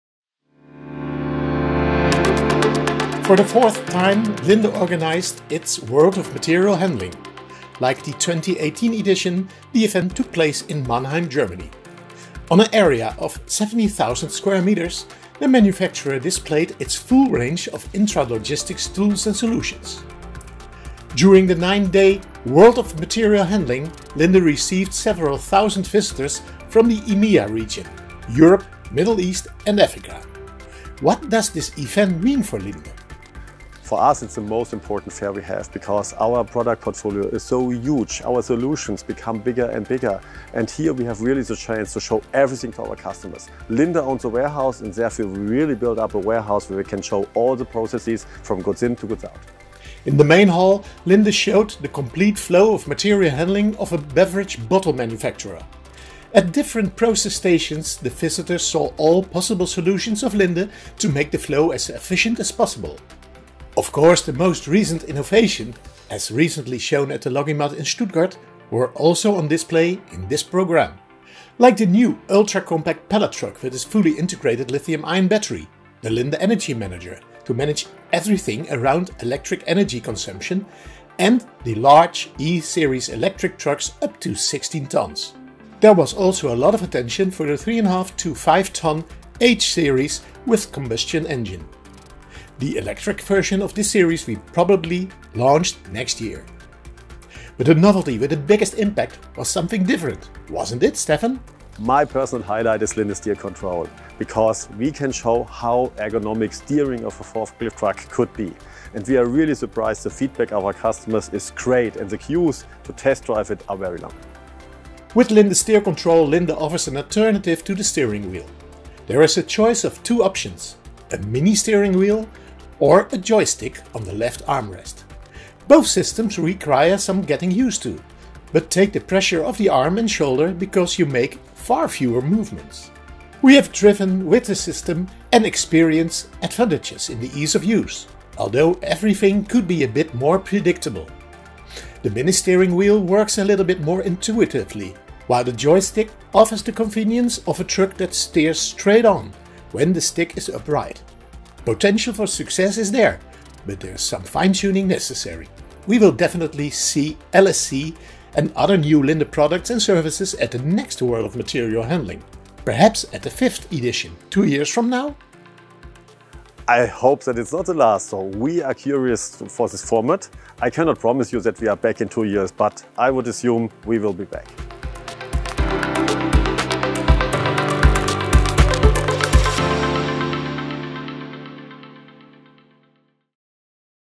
Linde introduces LSC at World of Material Handling 2022
On June 20, Linde Material Handling (MH) literally laid out the red carpet for its customers and other interested parties for nine event days. On an area of 17,000 square meters at the Mannheim Maimarkt site, the intralogistics specialist is presenting its extensive portfolio of products, solutions and services – for example, using the case of a bottle manufacturer to demonstrate the process from goods receipt to the last mile in the store. In addition, the vast expertise of the company’s product experts and consultants is available to visitors. Several thousand guests are expected at the World of Material Handling (WoMH), mainly from Europe. They all will see LSC, Linde Steer Control as a word first. No more steering wheel in the cabin of a forklift truck.